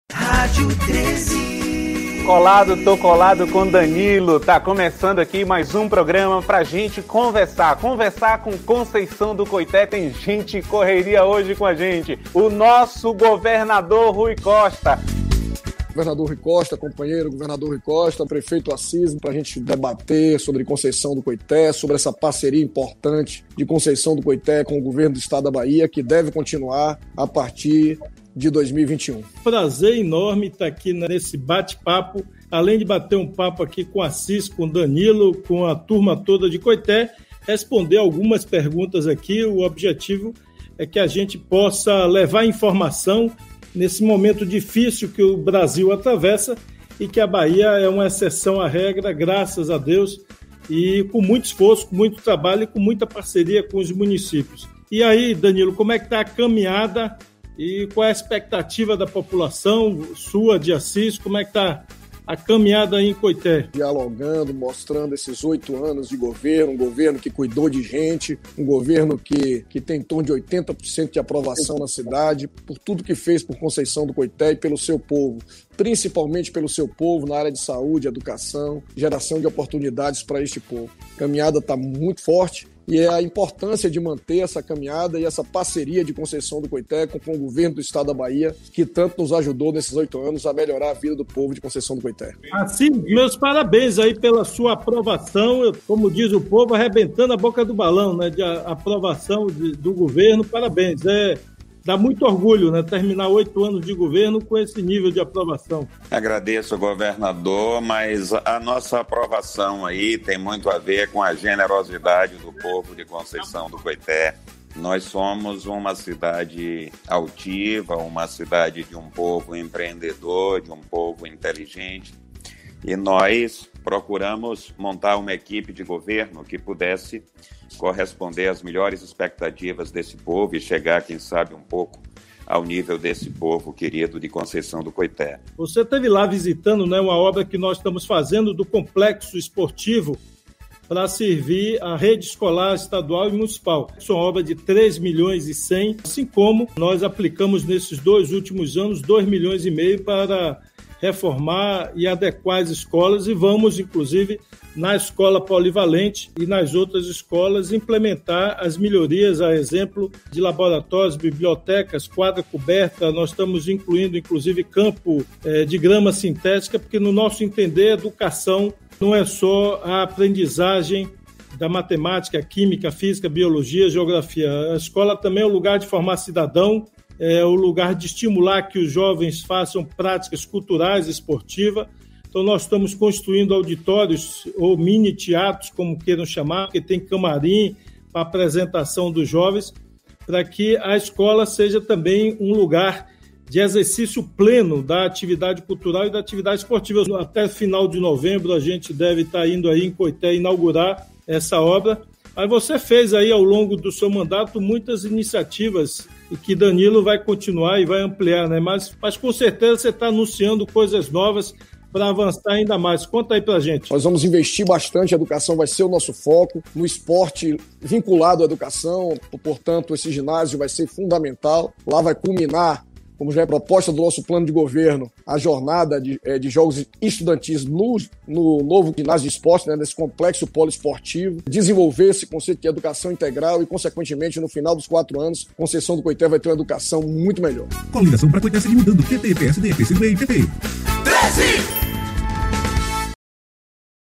Programa de Rádio n. 32: Danilo bate papo Rui Costa e Assis sobre Complexo Esportivo Educacional uma
Programa de Rádio n. 32: Danilo bate papo Rui Costa e Assis sobre Complexo Esportivo Educacional uma obra de R$ 3,1 milhões